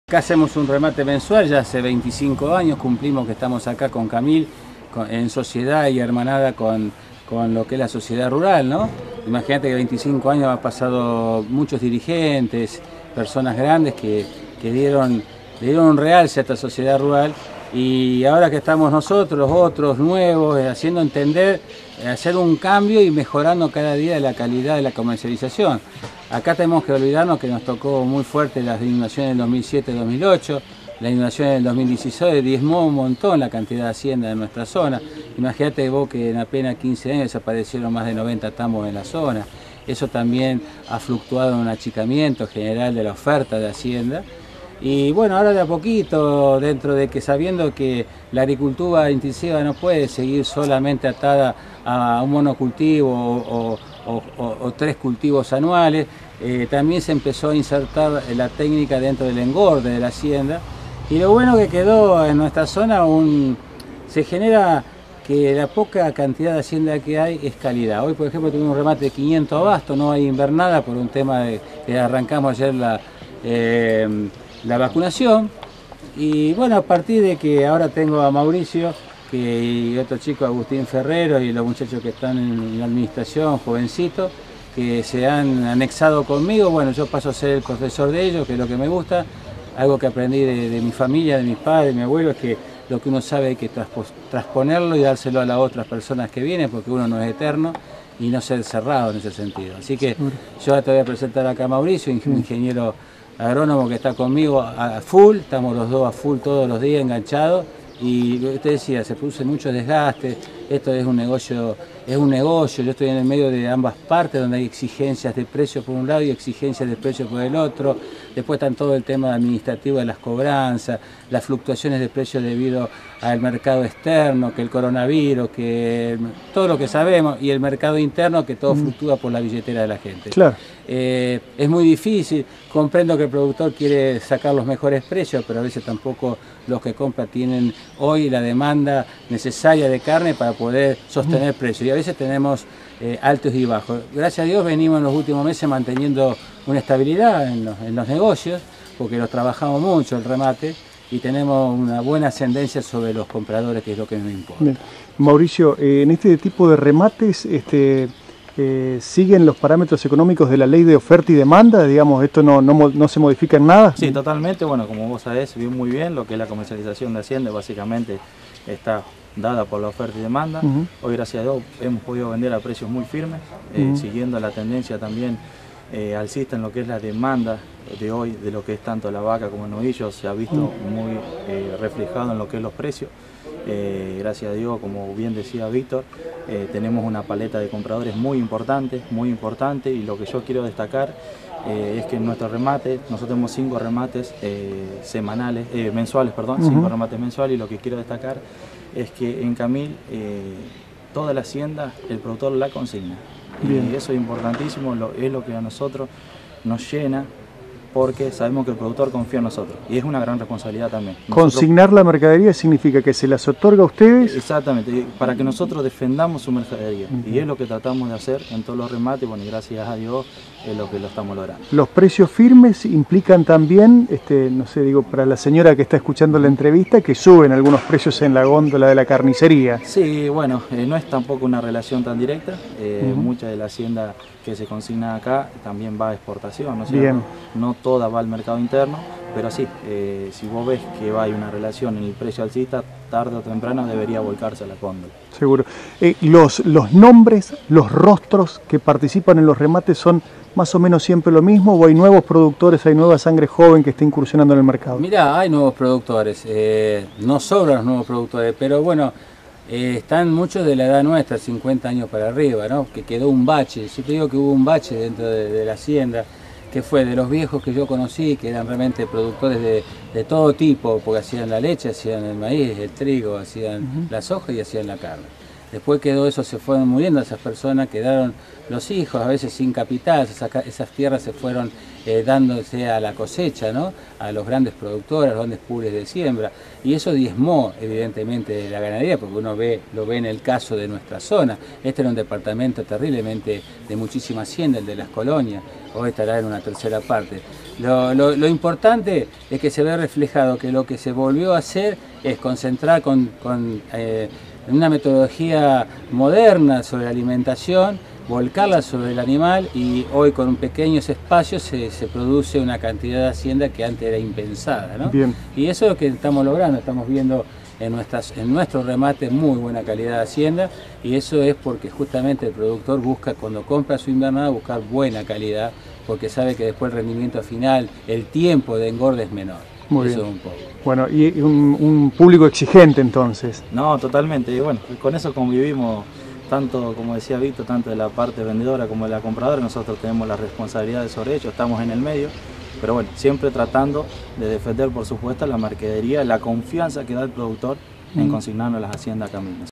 Víctor Cavallero - Martillero Público Ing. Mauricio - Camil
Se realizó el remate en las instalaciones feria de La Rural